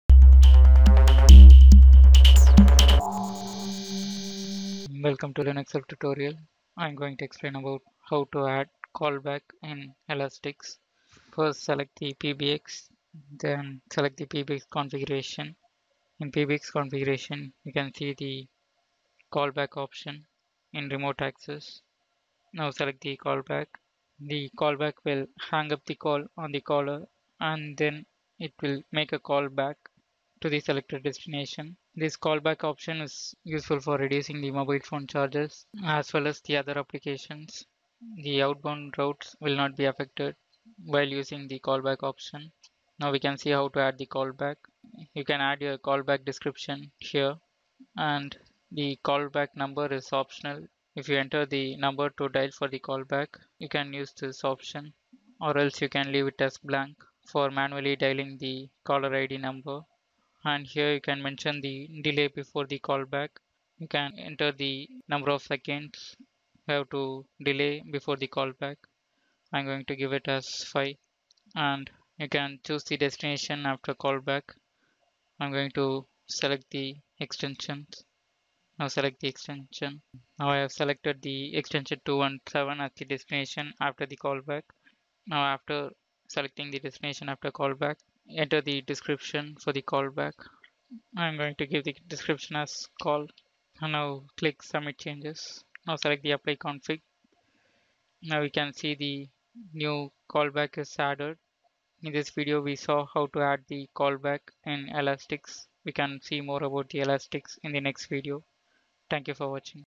How to add Callback in Elastix
This video explains the method to add Callback option in Elastix. Elastix is an open source software for PBX Unified Communication Solution.